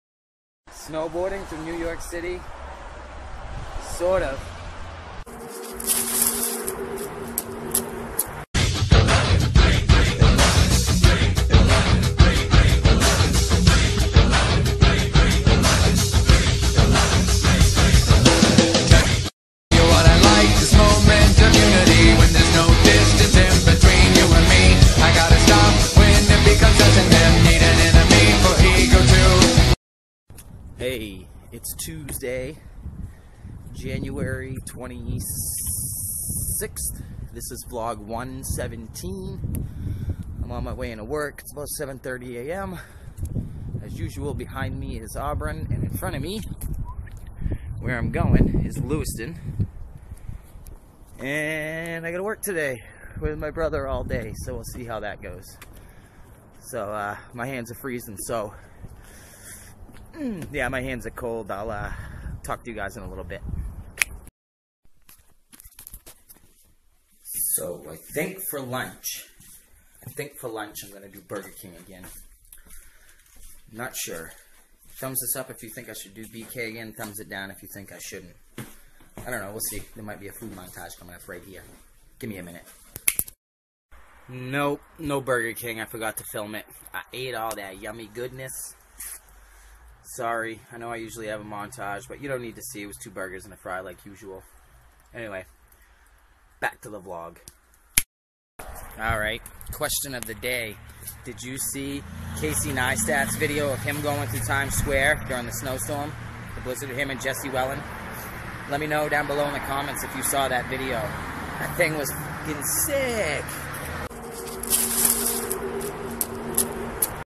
Snowboarding thru Times Square(sorta) vlog #117
Vlog #117 my daily vlogger vlog.
Question of the day:
"Did you see Casey neistats snowboarding thru NYC video"?